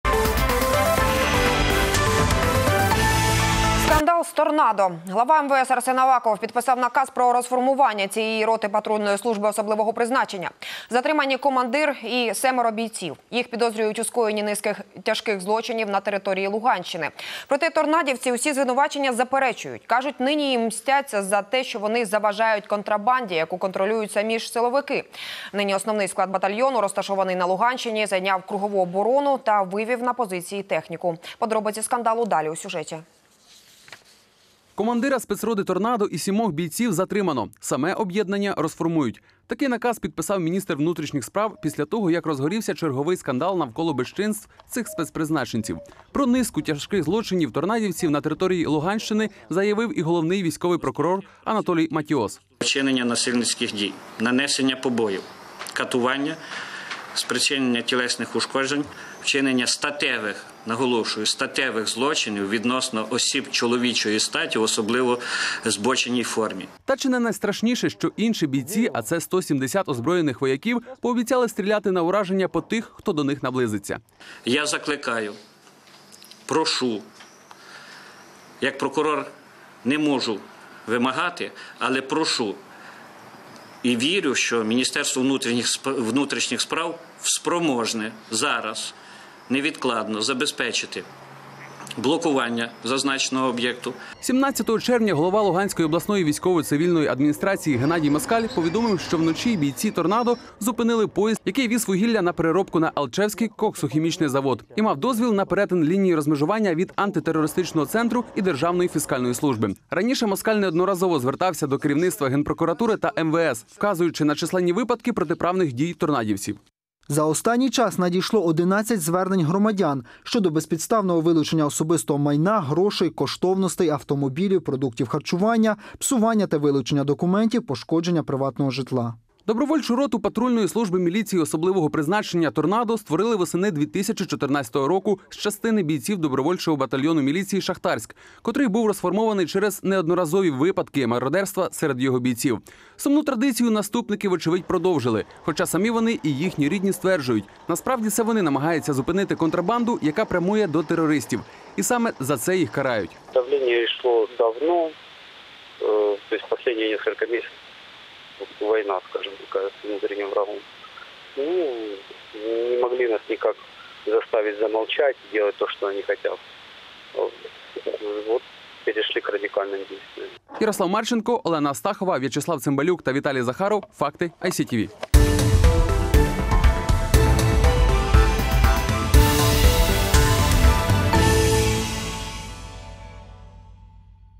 170 бойцов «Торнадо» захватили школу и угрожают стрельбой
Бойцы роты Торнадо, которую глава МВД поручил расформировать, закрылись на своей базе в помещении школы в Луганской области и готовы использовать оружие.

Бійці роти Торнадо, яку глава МВС доручив розформувати, закрилися на своїй базі в приміщенні школи в Луганській області і готові використовувати зброю.